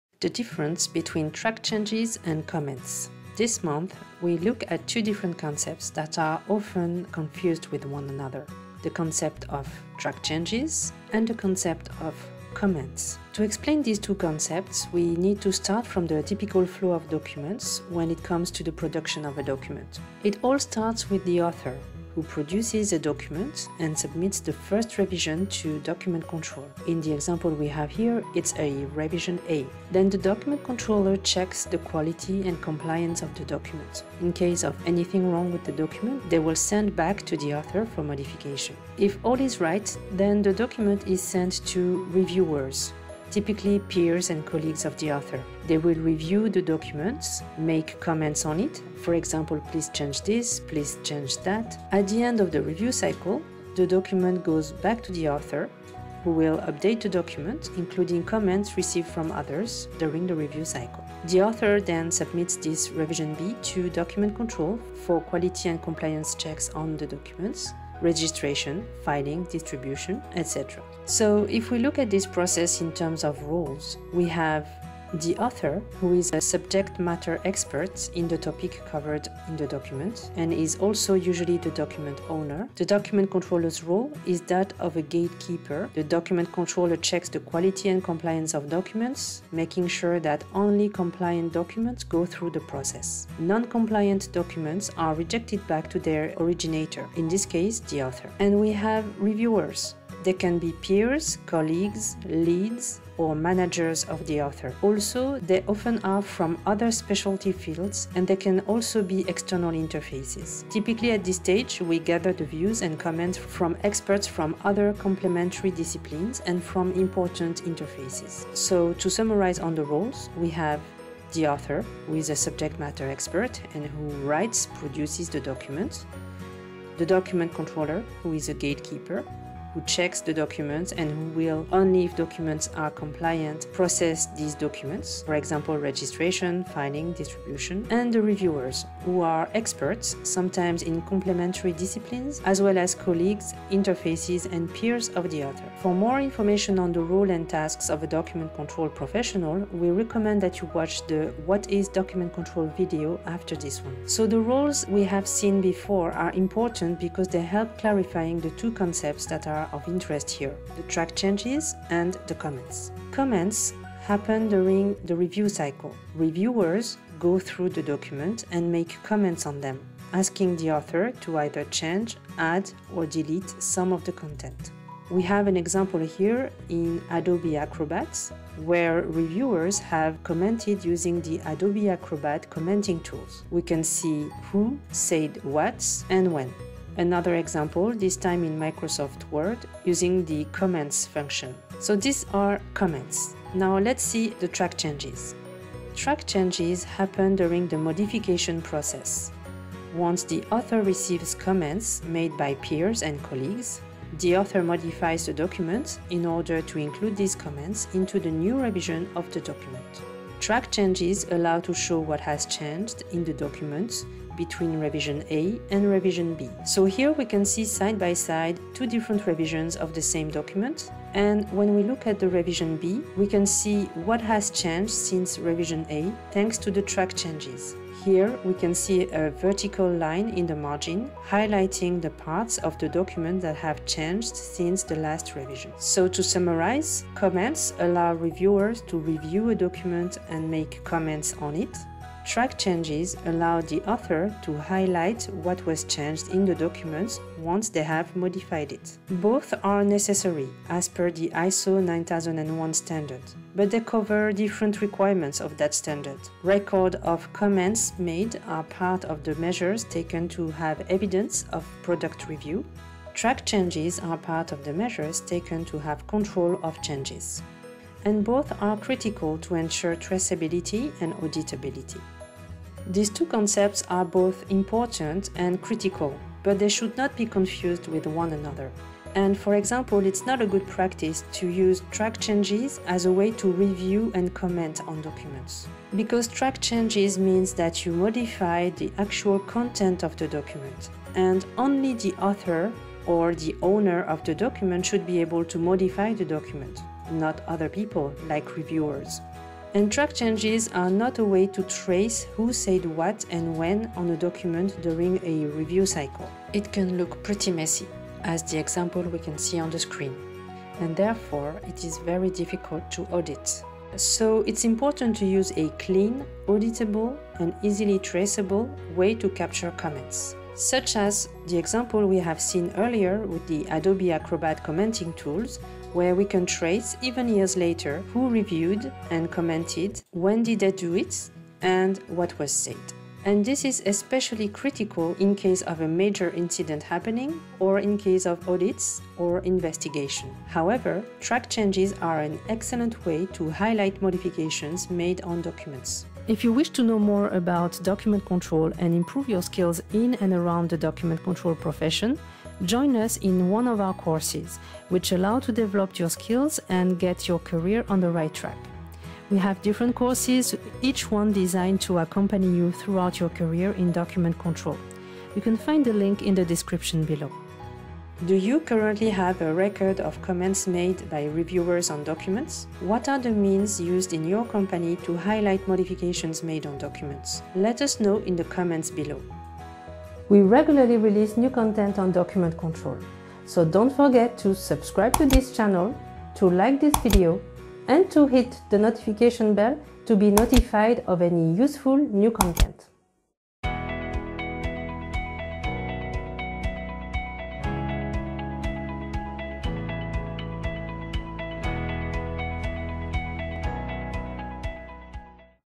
What is the difference between “track changes” and “comments” [Consepsys Tip of the Month]
This informative video explains two different concepts that are often confused with one another: The concept of track changes and the concept of comments.
This video is part of a series called "Document Control Tip of the Month" presented by Consepsys, the World Leaders in Document Control